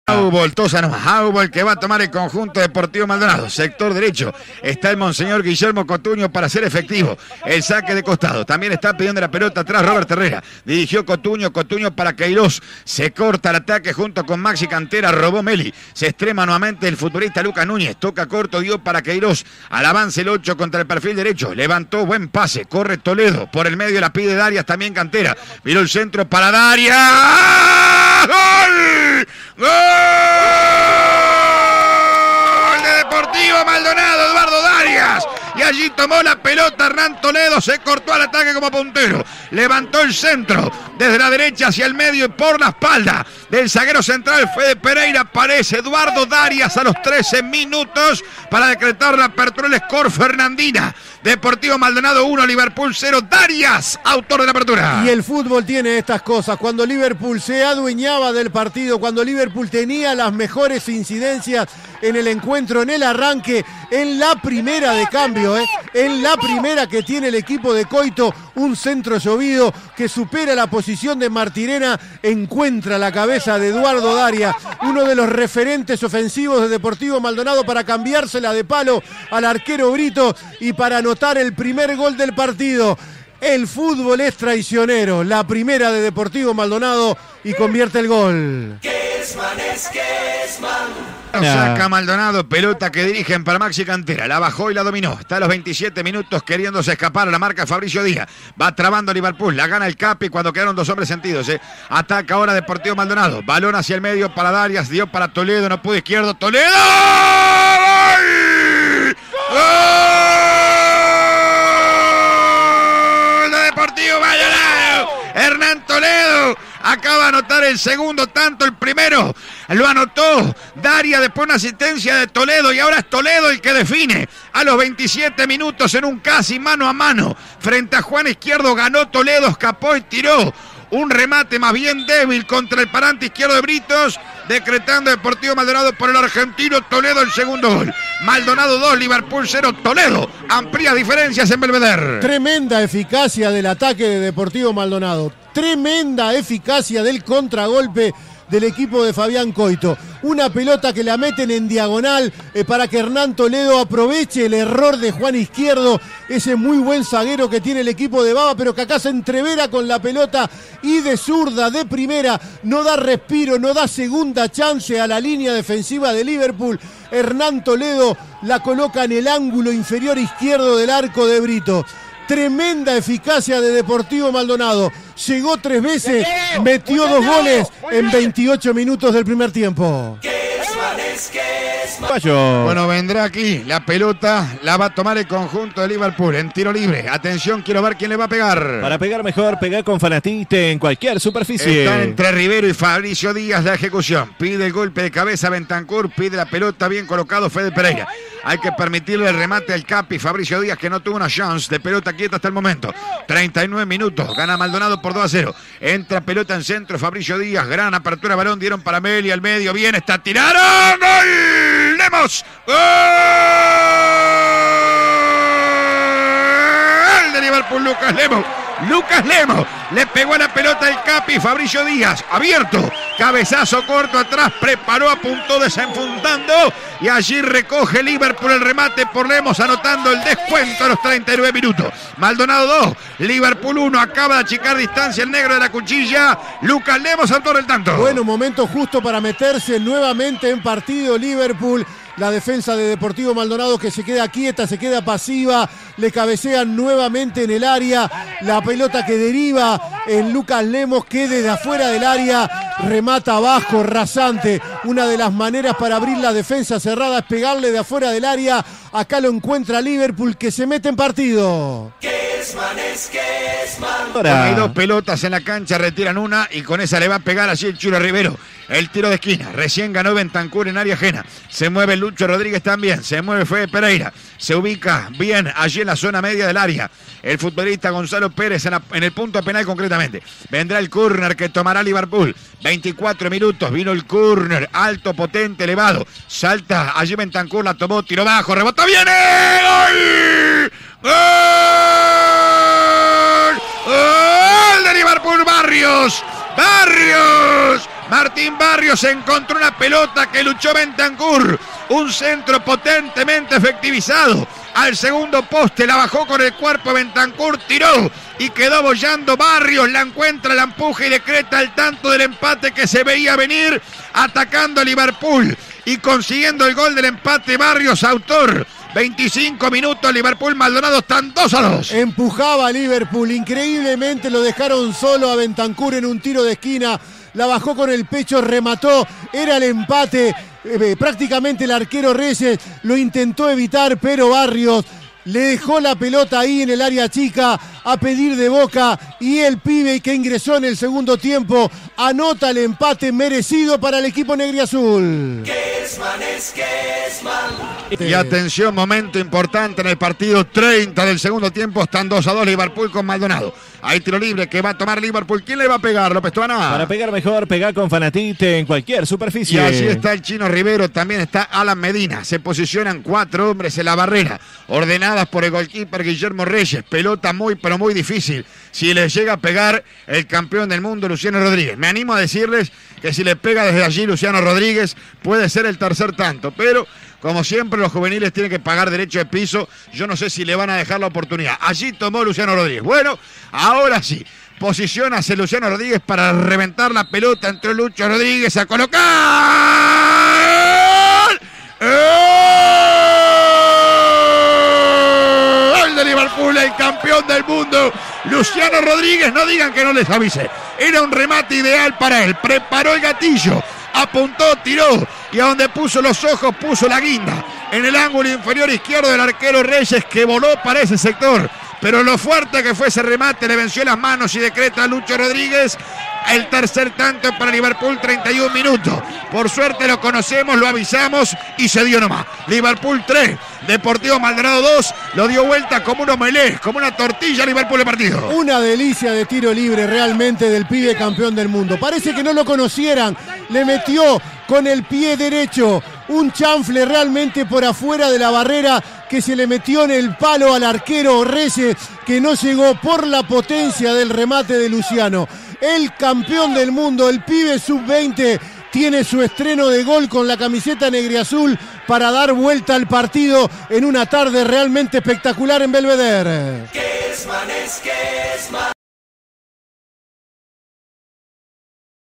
Liverpool 3 Deportivo Maldonado 2 - Intermedio 2023 (por Martín Kesman)
Los goles del partido, por Martín Kesman y Gustavo Ricci para CX 22 Radio Universal 970 AM.